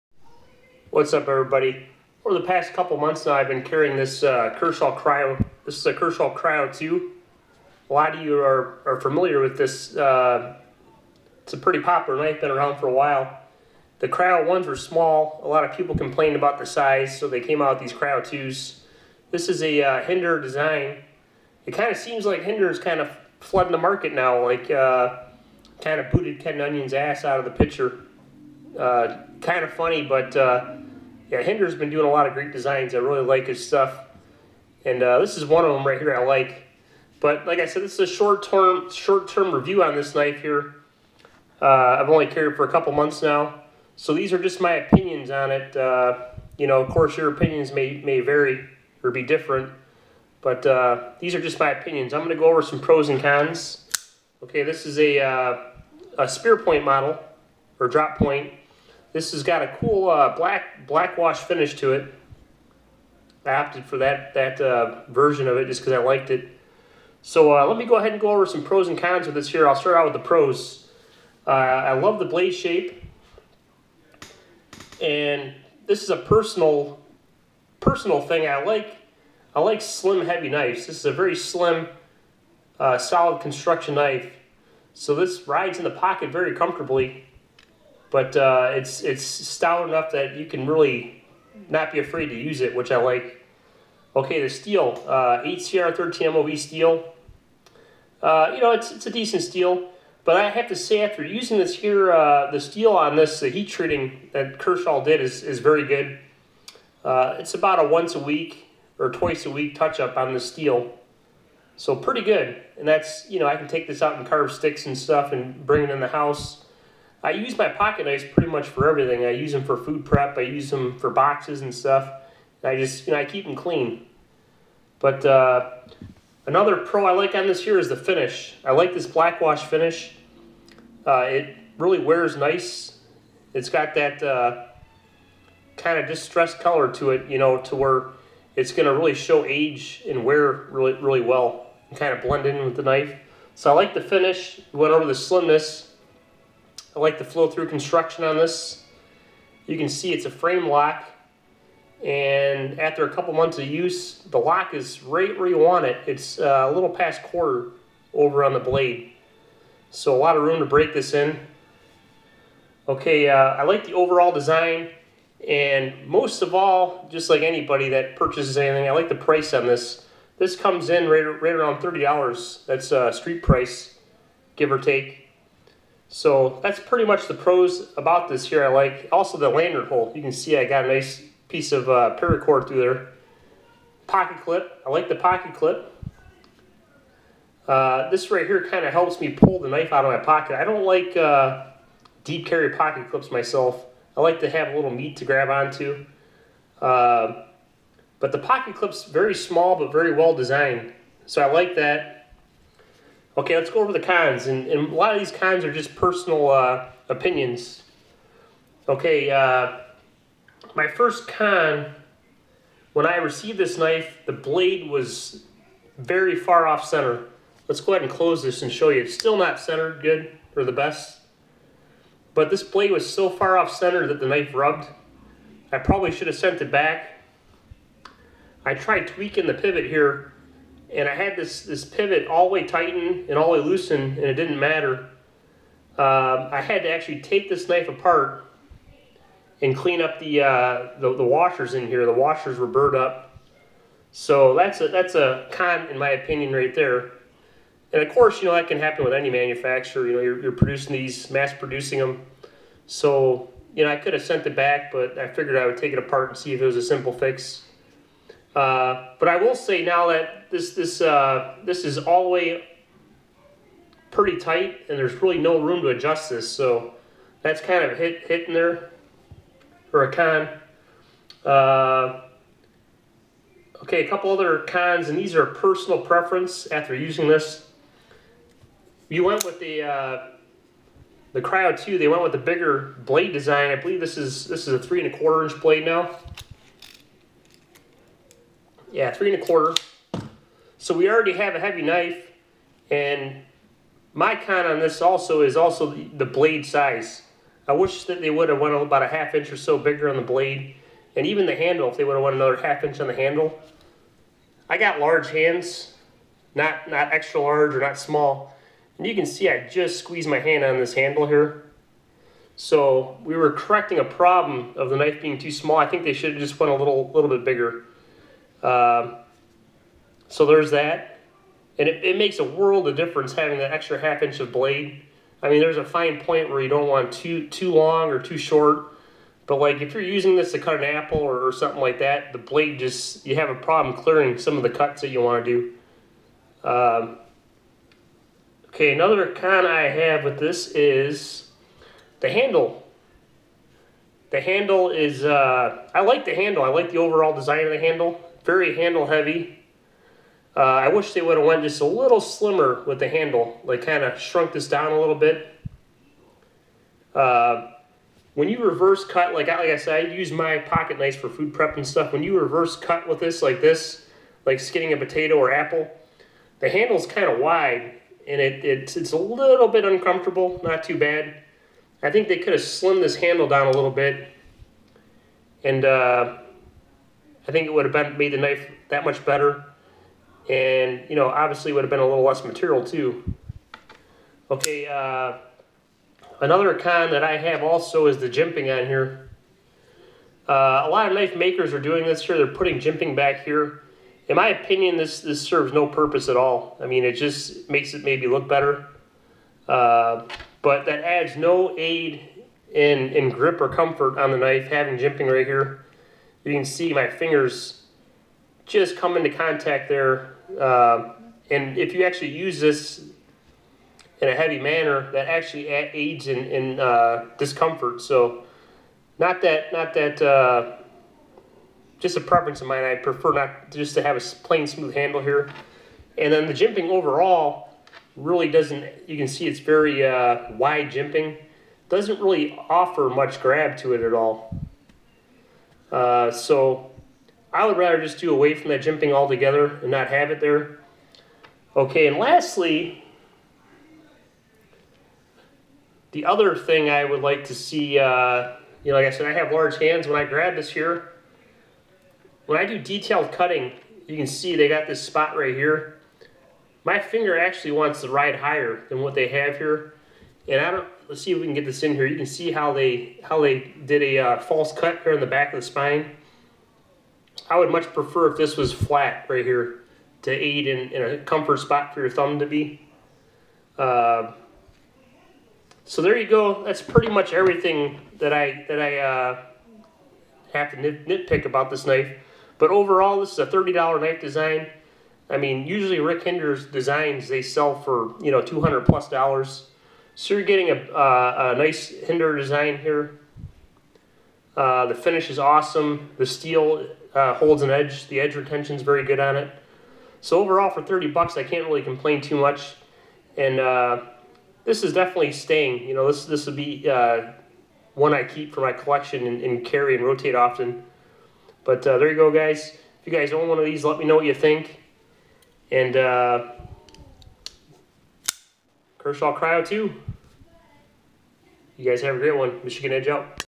Kershaw Cryo 2 review and thoughts
The Kershaw 1556BW Cryo 2 in black wash finish has been my edc for the past couple months.  Just a video sharing my thoughts on this knife.  Designed by Mr Hinderer. I give this knife 4 out 5 stars overall. For around the $30 price tag it is a great edc option.